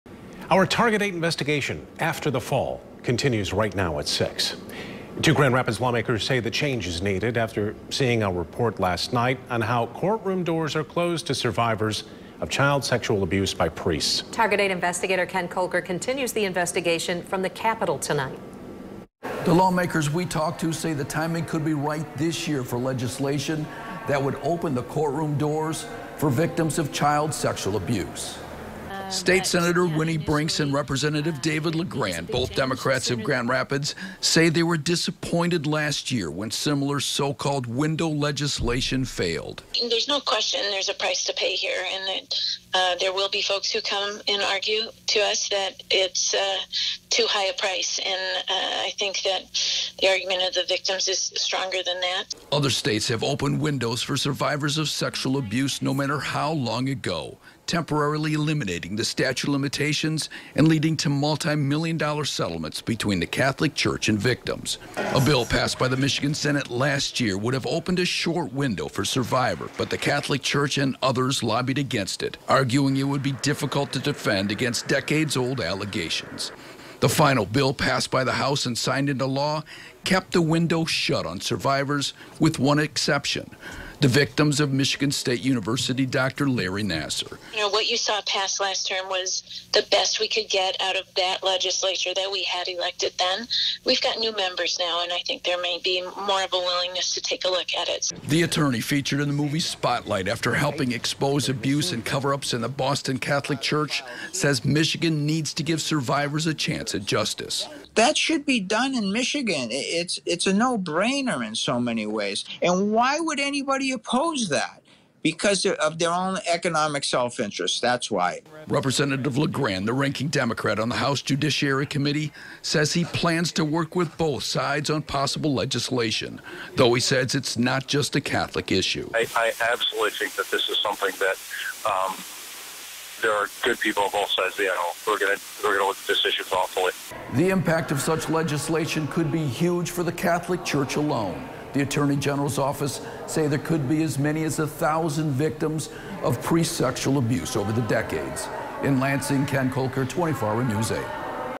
Lawmakers: Investigation shows need for change
Two Grand Rapids state lawmakers said the timing could be right this year for legislation that would open courtroom doors for survivors of child sexual abuse at the hands of Catholic priests and others. (April 26, 2019)